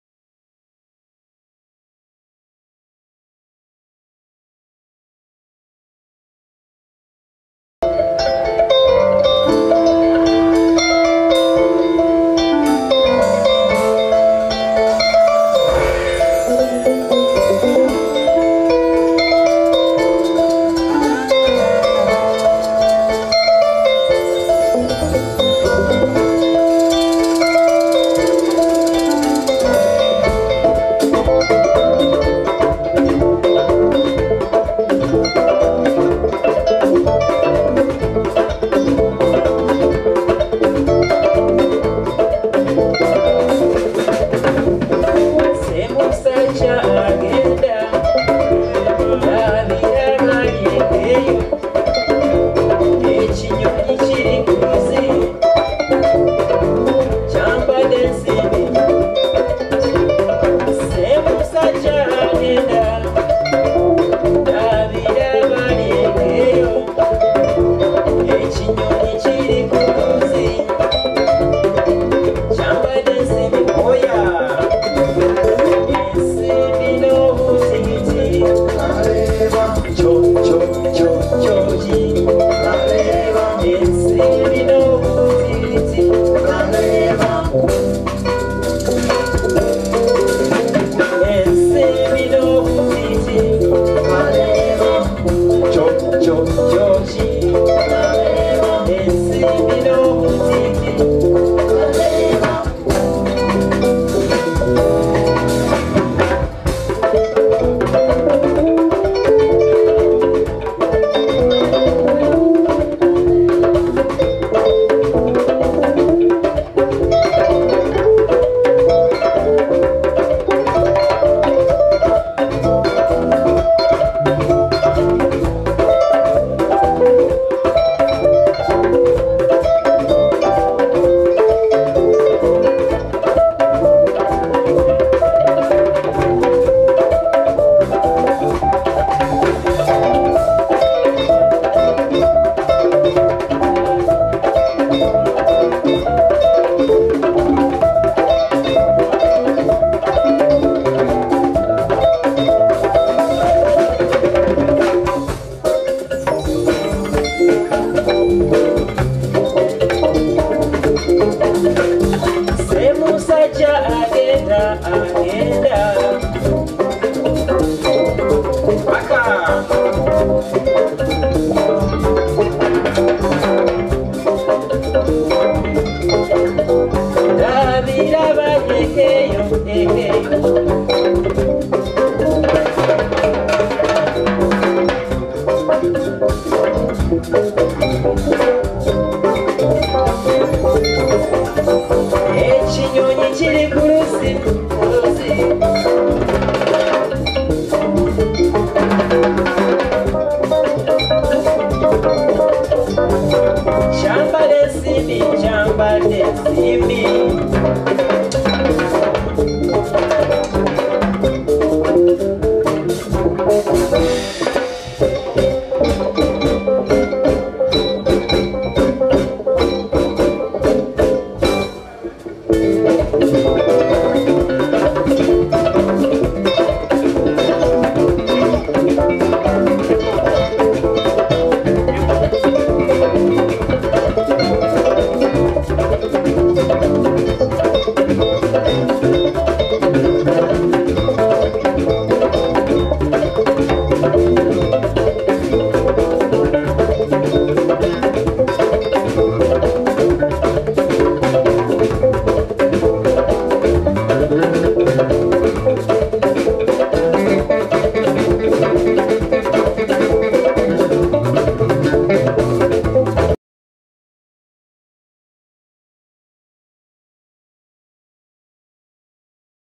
Martin w Soul Beat Kaleeba
Martin Klabunde plays djembe with Kinobe and Soul Beat
Africa in Kampala, Uganda.New Years Eve 2007